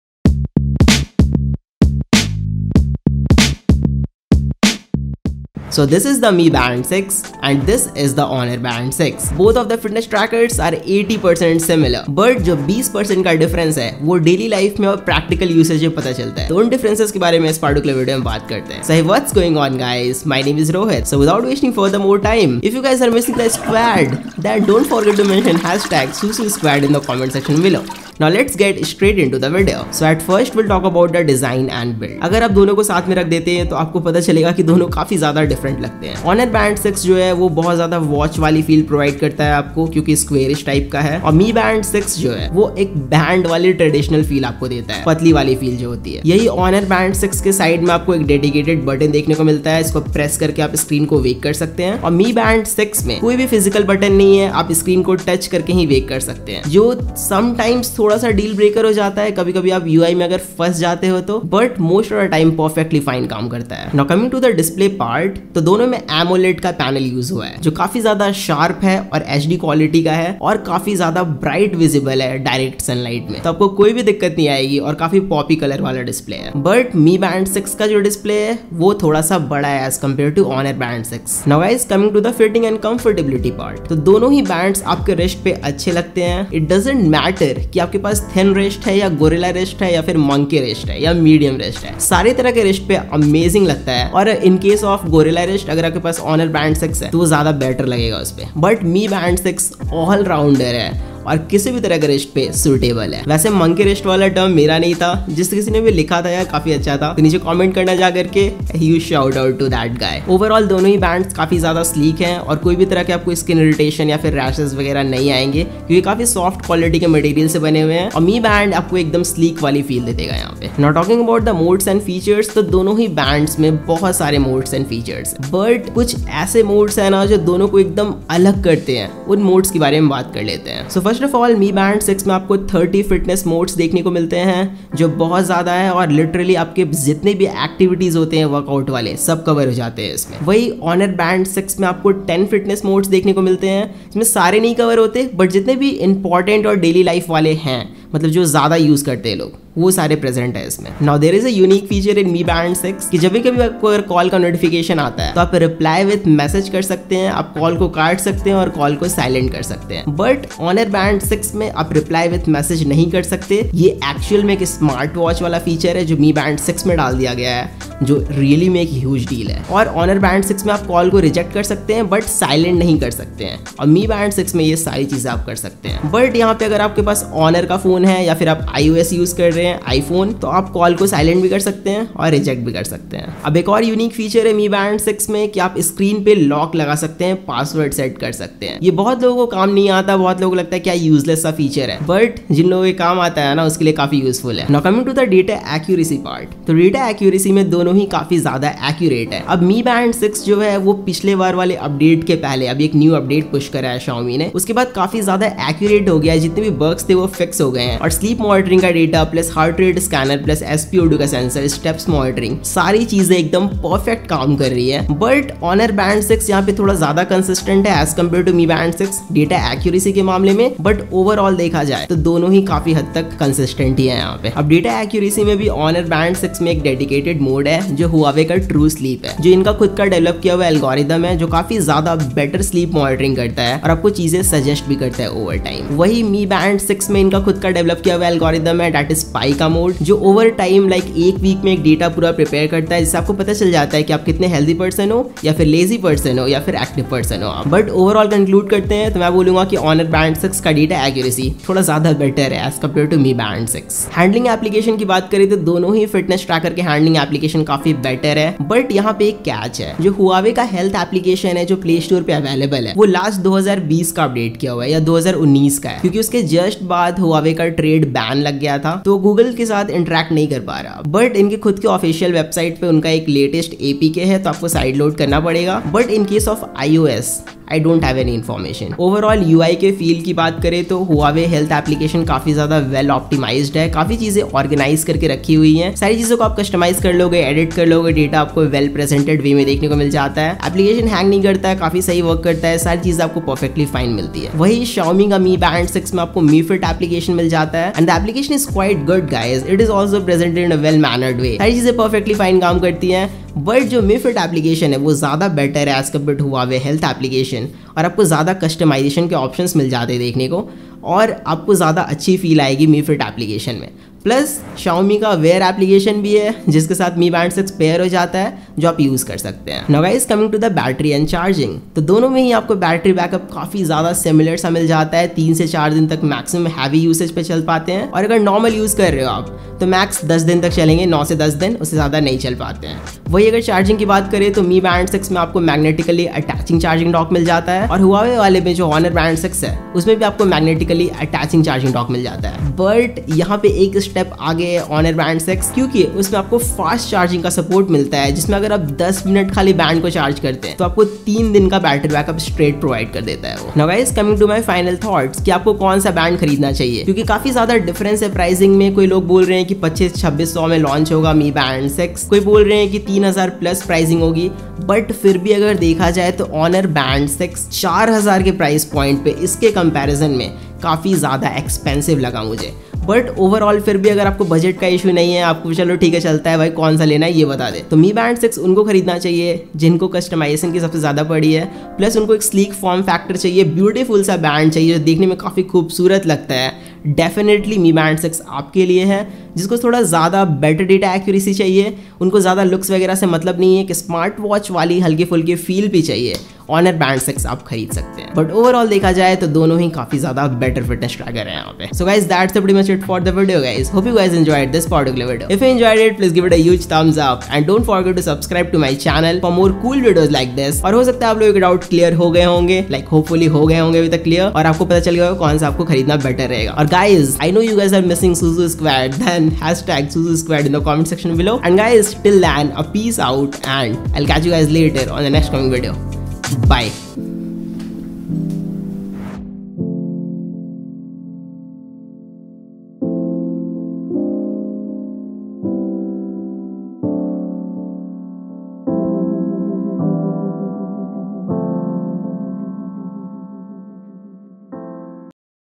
Mi Band 6 Vs Honor Band 6 | Which One To Buy?
Mi Bands 6 and Honor band 6 are the most amazing fitness trackers out there in the market as of now, but both of them gives a tough competition to each other so in this video, i have compared the MI band 6 and honor band 6, so that you guys can decide which one to buy.

HUAWEI Band 6

00:00 Intro
00:30 Design And Build :-

Both totally different
Honor band 6 feels like a watch
Mi band 6 feels like a band

Honor band 6 has a dedicated button
Mi band 6 don't have physical button

01:04 Display :- Both have AMOLED panel but mi band 6 has a bigger display

01:23 Fitting And Comfortability :-

Both looks good on any type of wrist whether it is a gorilla wrists or medium or thin wrists or monkey wrists.

Overall mi band feels more sleek on hand.

02:02 Modes And Features :-

Mi band 6 has 30 Fitness modes

Honor band 6 has 10 fitness modes

In mi band 6 you can reply with messages to call notifications even u can silent the calls.

In honor band 6 u can only disconnect calls but incase if you have ios device then u can silent the calls.

Mi band has password lock function screen lock

03:16 Data Accuracy :-

Both are pretty accurate in Heart Rate, SpO2, Steps, Sleep monitoring
Mi band is accurate after the update

Dedicated Huawei true-sleep in honor band 6

Dedicated PAI mode in Mi band 6

Honor band 6 is slight better in data accuracy

04:15 Handling Application :-

Both apps are well organized and optimized but huawei health latest app is not on playstore , can be downloaded separately.

Mi fit have more customization options and xiaomi wear app as well.

05:21 Battery And Charging :-

Both have magnetically attaching charging dock.

Both have similar battery backup of 3-4 days on heavy usage and max9-0 days on normal usage.

Honor band 6 comes with fast charge support that can charge ur band in 10min for 3days.

05:55 Final thoughts :-

MI band 6 for more features and customization.

Honor band 6 for more better data consistency and accuracy.

User Interface :-

Both bands have smooth user interface and responsive touch gestures.

Dedicated video on both bands check the card
Heyy this is Rohit and on this channel i do amazing Apps,Photography,Smartwatches,Smartphone Honest Review.